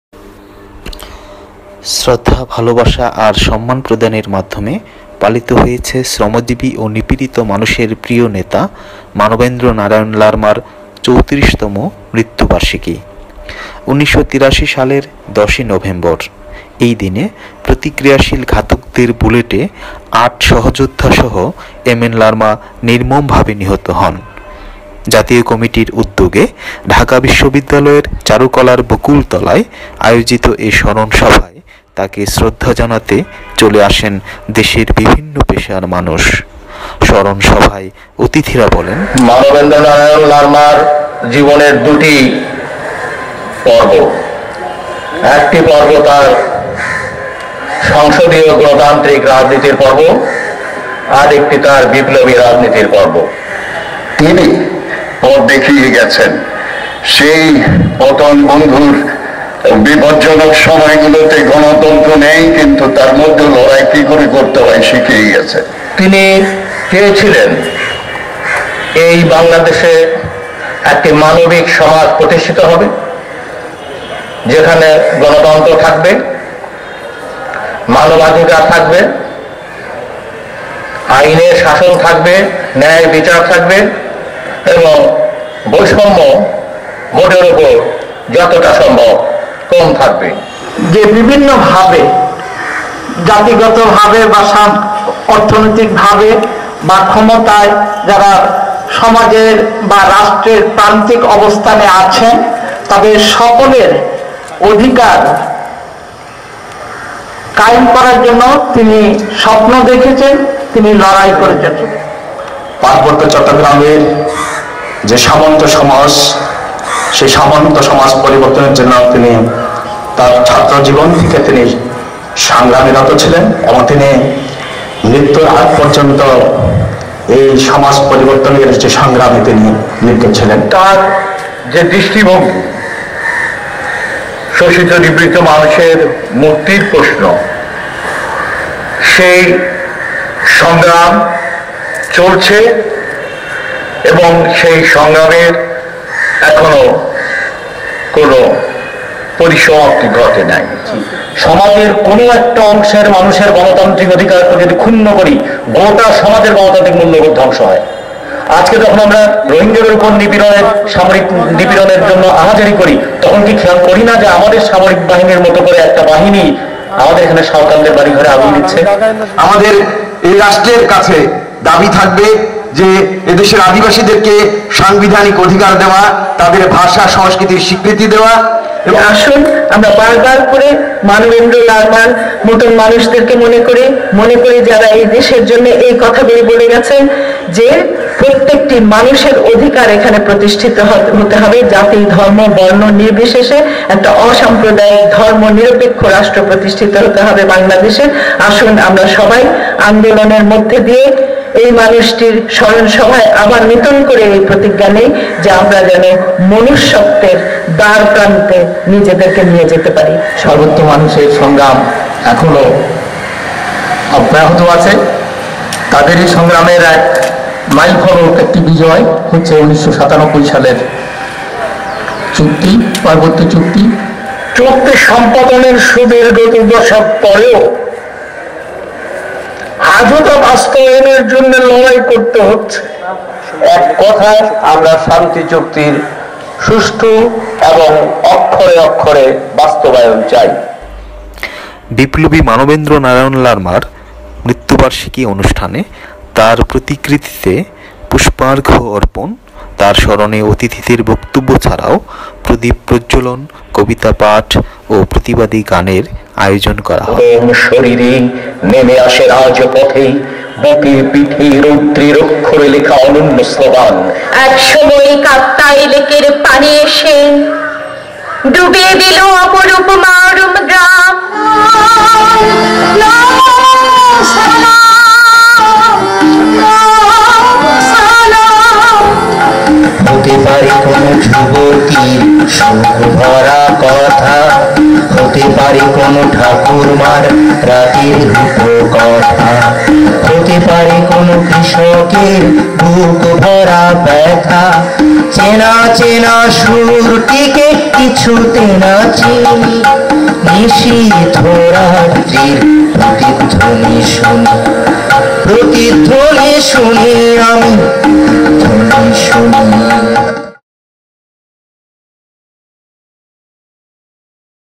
Respect to MN Larma 2017 (10 November)
বিপ্লবী নেতা মানবেন্দ্র নারায়ণ লারমার ৩৪তম মৃতু্যবার্ষিকী পালন, ঢাকা, ১০ নভেম্বর, ২০১৭